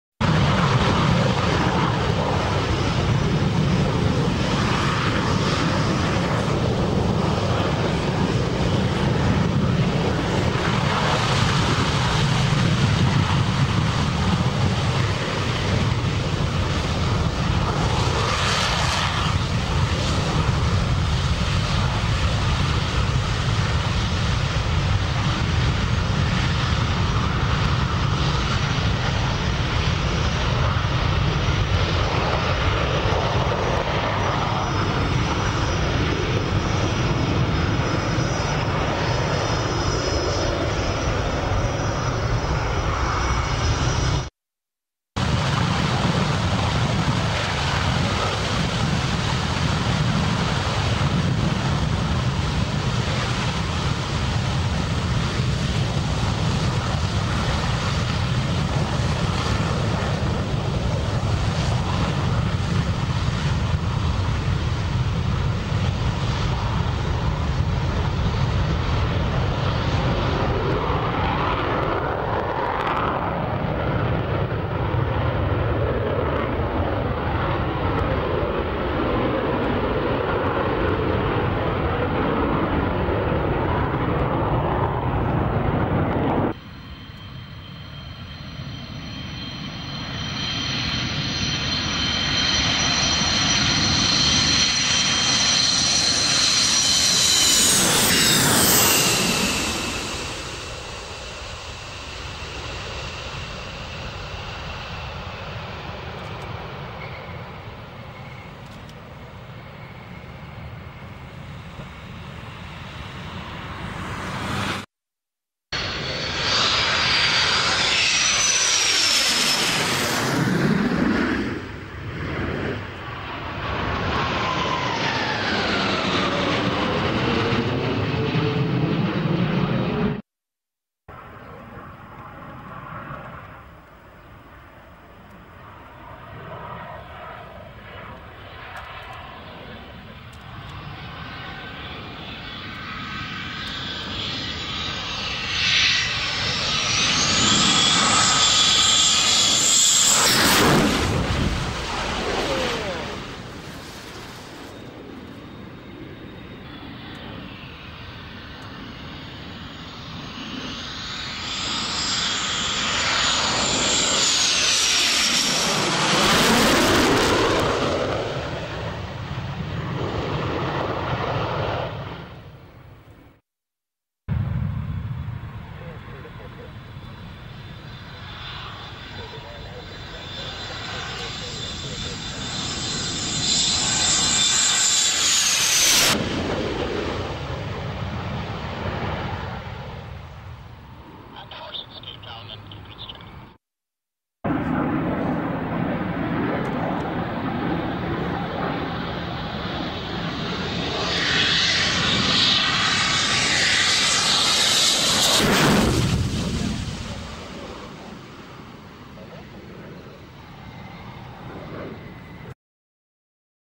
"LAST NOMAD" RAF WADDINGTON 2004 filmed in hi8
The last ever "NOMAD"excercise held at raf waddington in 2004.
A full dvd of the event is on sale together with the acmi dvd for the qra shed appeal by the lightning preservation group.
anyone interested please send me a private message
THIS VIDEO IS UNDER COPYRIGHT TO JETNOISEFOREVER.